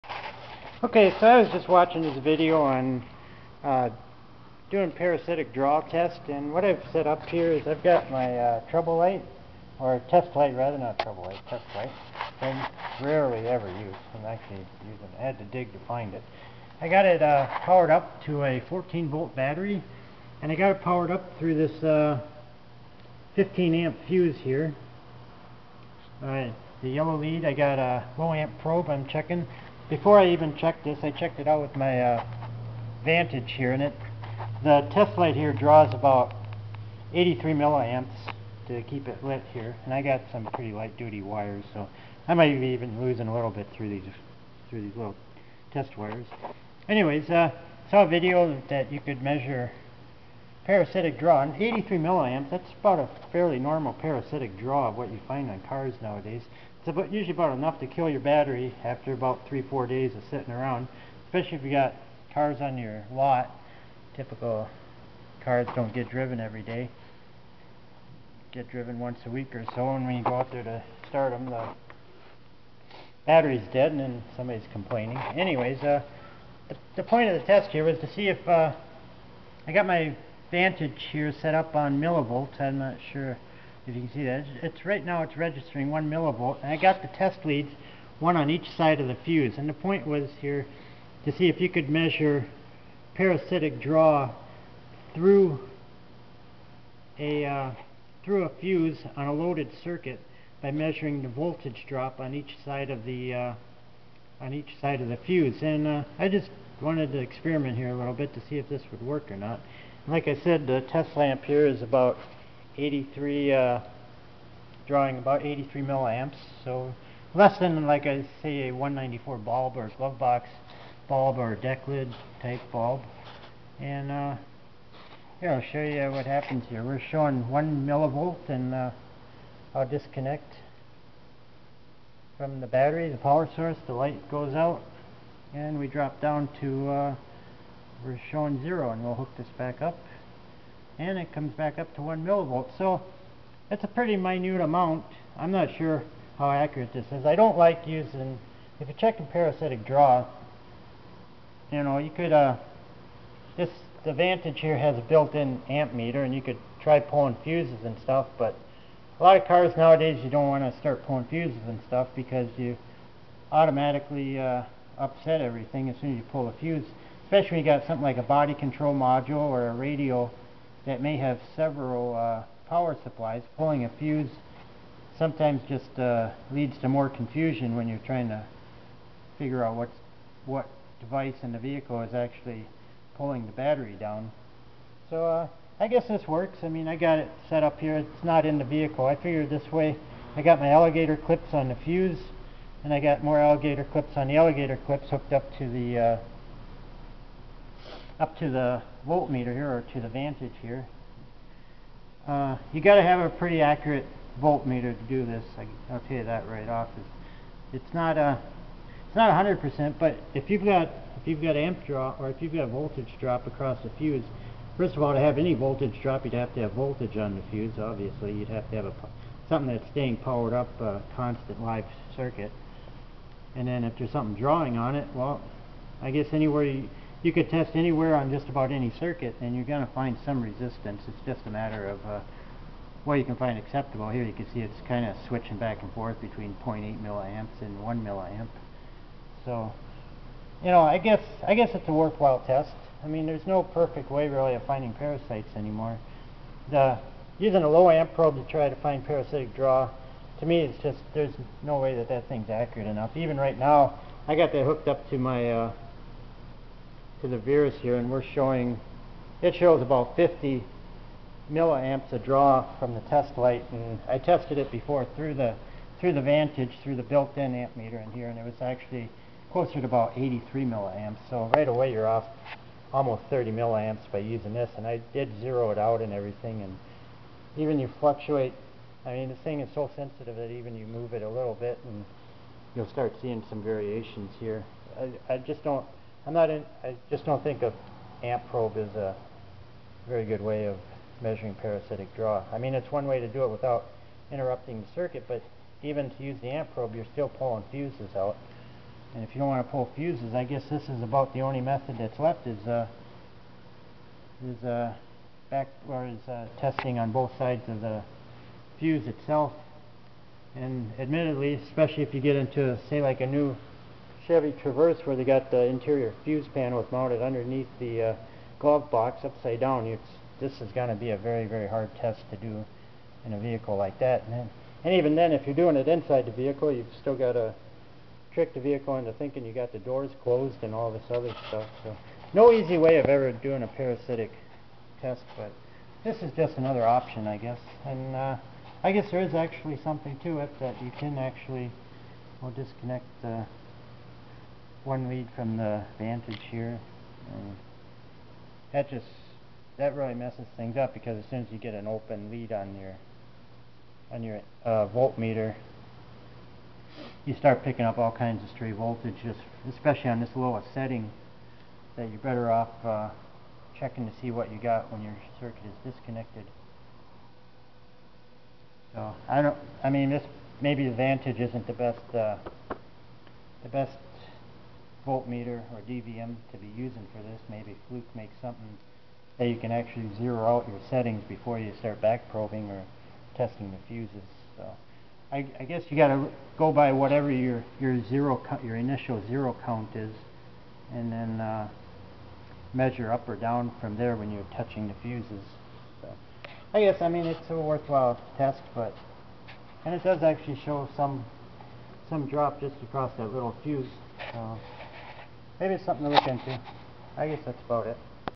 Checking parasitic draw
Using a voltage drop test on a fuse to check parasitic draw. The problem with the Vantage is it picks up too much interference when the leads are not connected to anything as seen at the end of the video. Other than that this seems to be a valid method of testing for a loaded circuit that might be drawing your battery down.